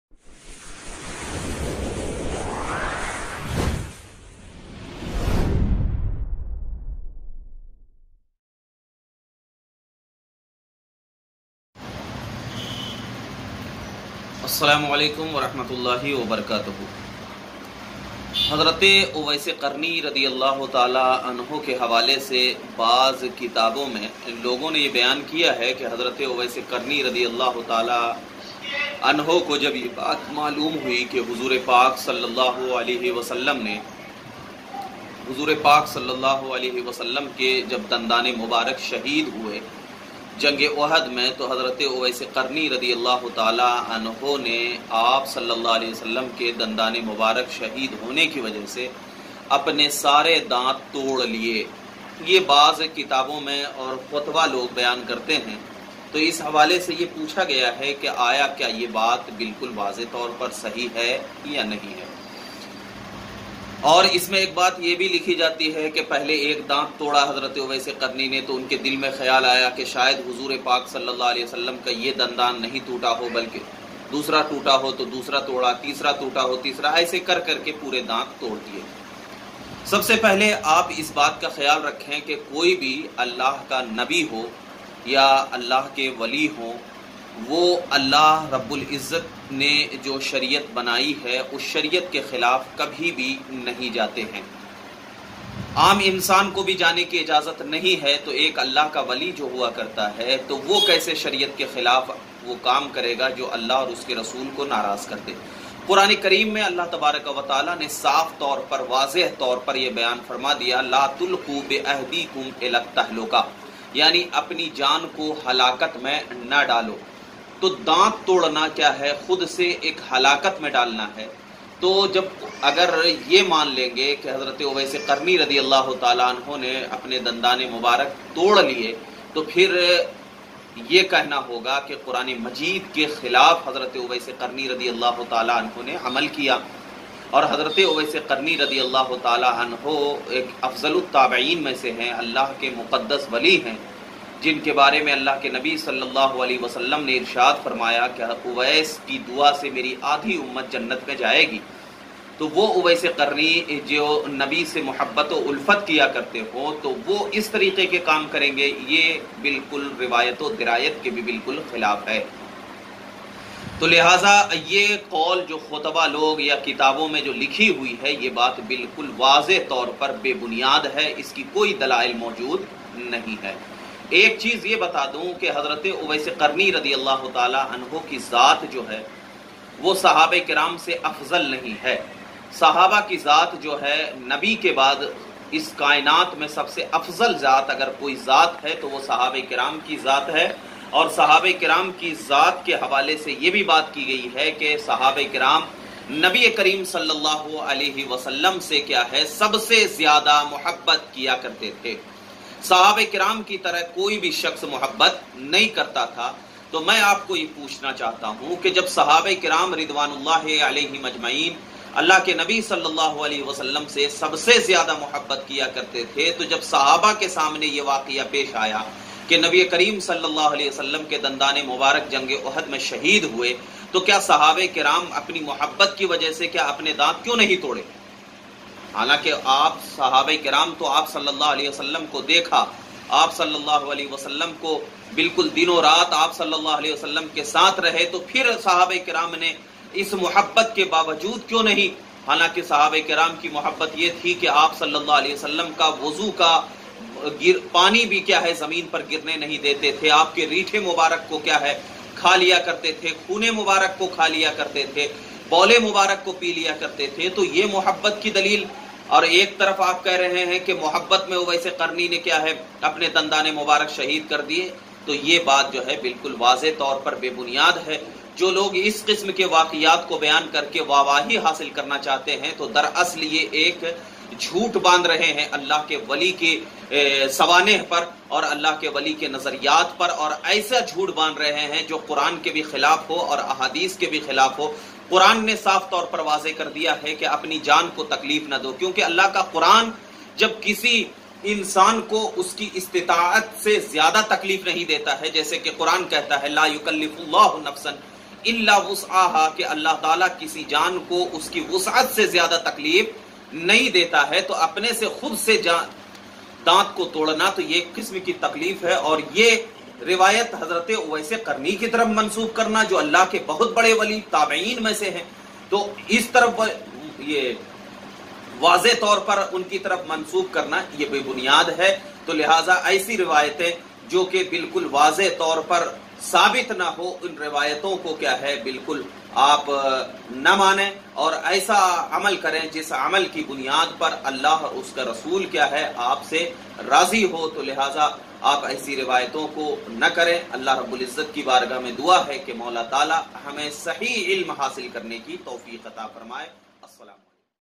Kya Hazrat Owais Qarni nay Apne Daant Tor diye thay by Mufti Monauwar Hussain Ashrafi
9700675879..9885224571